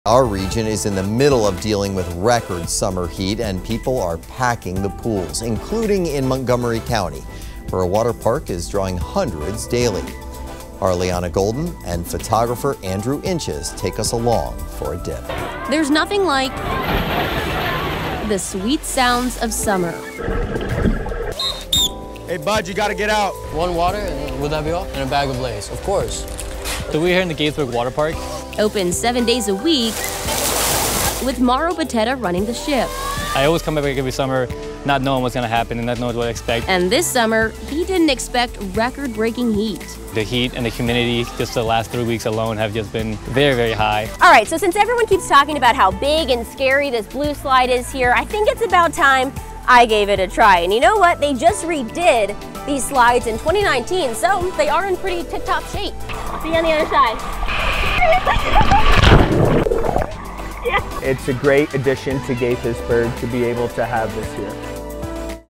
Hitting the pool to beat the heat
With temperatures in the area hitting the triple digits, pools in the area have been packed lately, including at this water park in Montgomery County.